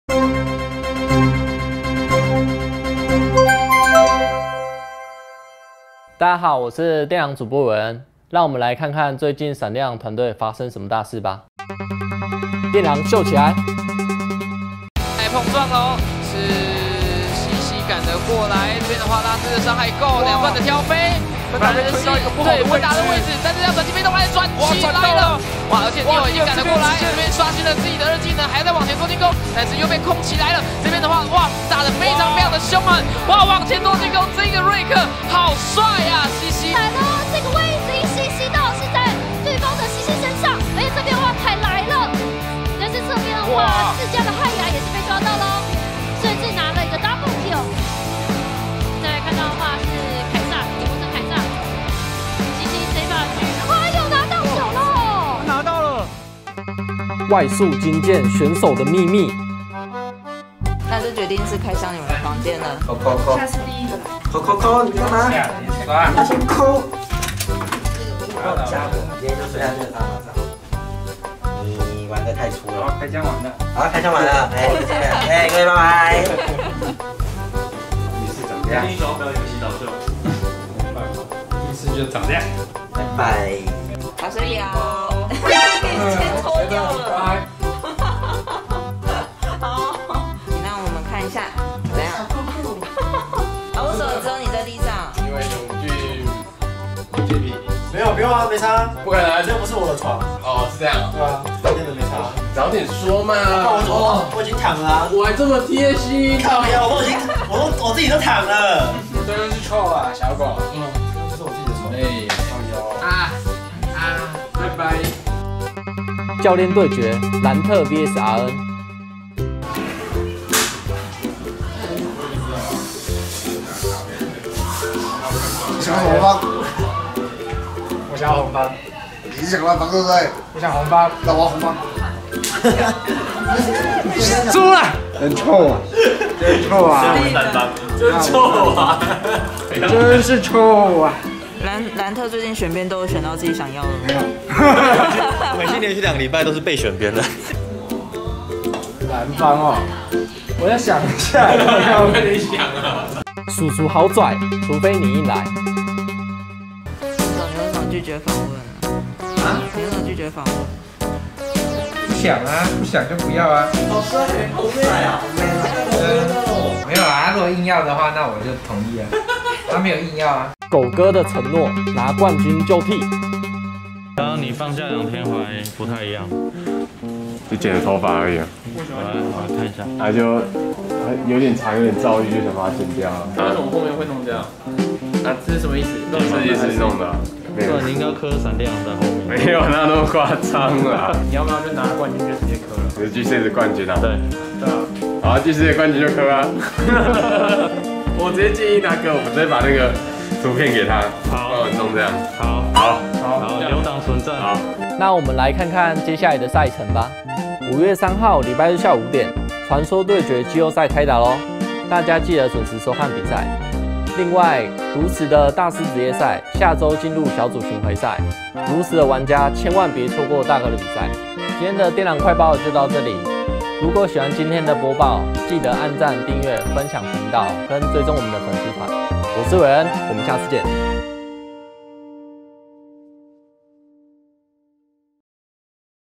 外宿驚爆 選手的秘密｜閃電狼 FW x 電狼快報
最期待的 電狼快報 來啦！
讓主播 FW Wayn帶你們開箱肖年狼外宿的秘密😳
-本週提要
▵電狼秀起來
▵外宿驚見選手的秘密
▵教練對決，蘭特VS RN
▵叔叔好跩，除非你硬來
▵狗哥的承諾，拿冠軍就剃！
▒ 閃電狼消息隨時追蹤 ▒